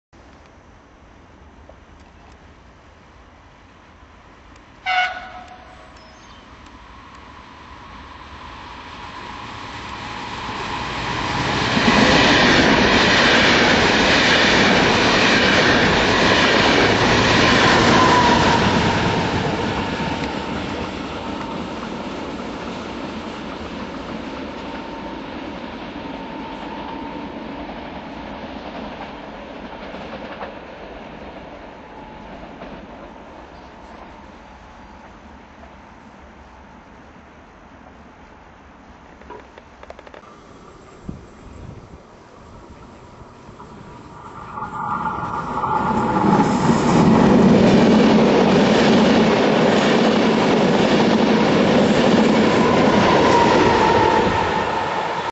Doppio video - vivalto
Treno Regionale 2212 per Venezia SL ripreso in due giorni diversi e in due posti diversi.
La prima parte del video è del 24 gennaio 2008 a Grignano (non ricordo il numero della E464), la seconda del 31 gennaio, circa 2 km più avanti (con E464 382).